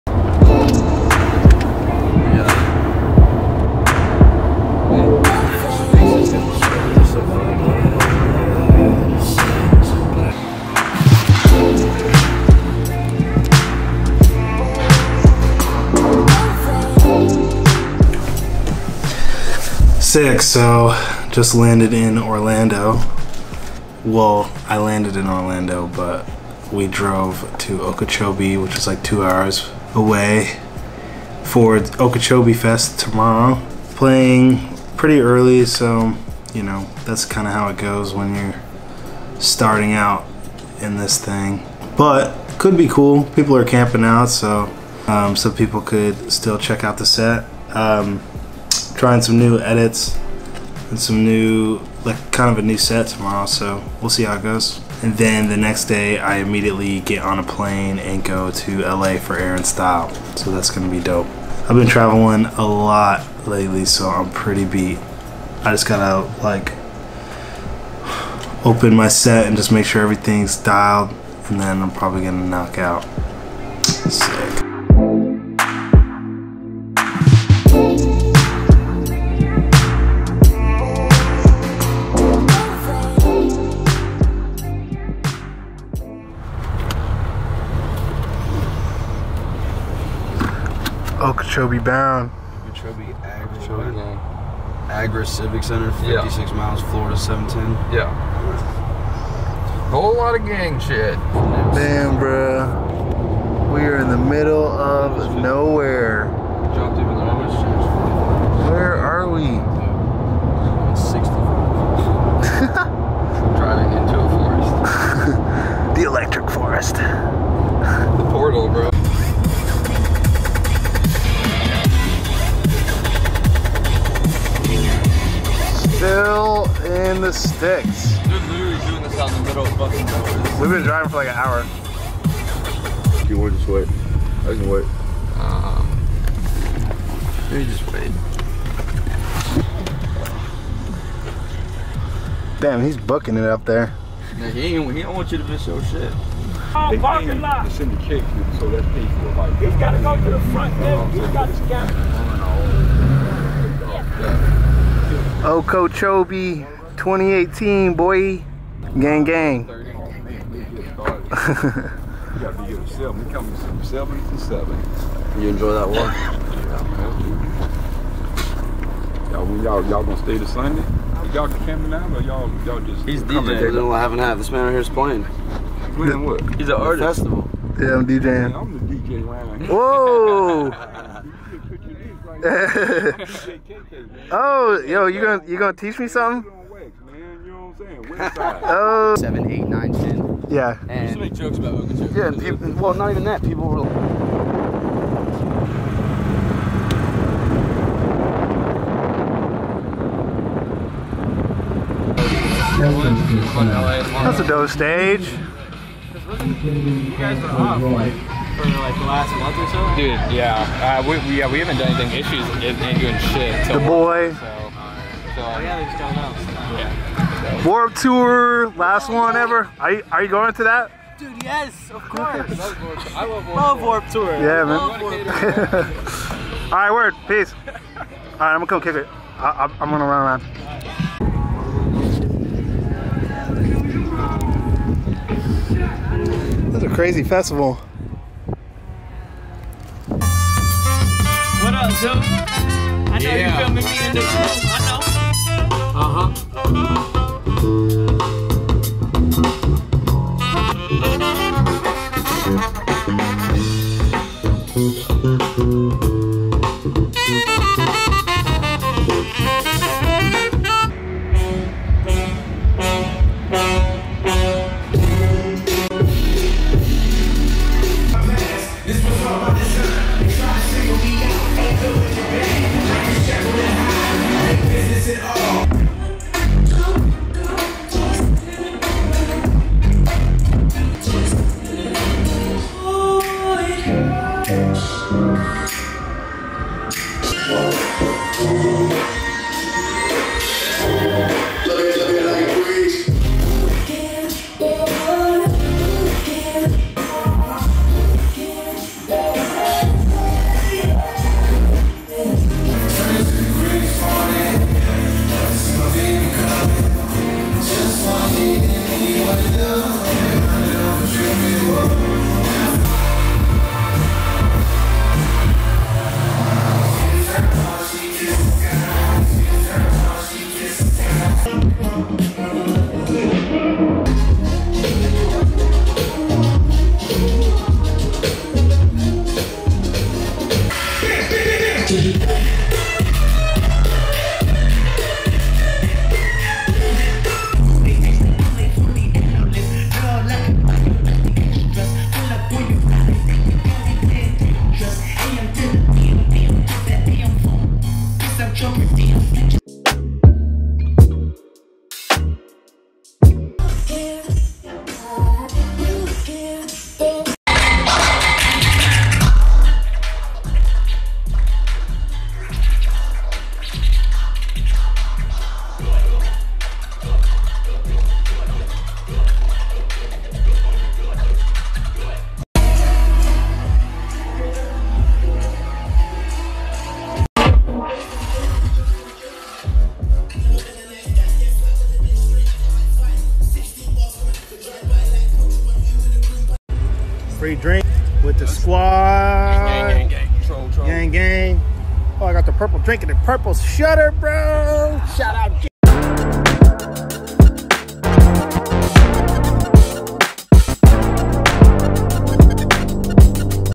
LO VLOG: MY OKEECHOBEE SET WAS SURPRISINGLY LIT
Didn't expect much from my early set time at Okeechobee, but the crowd grew and grew and turned out to be a solid day.

*all music used in this video is original*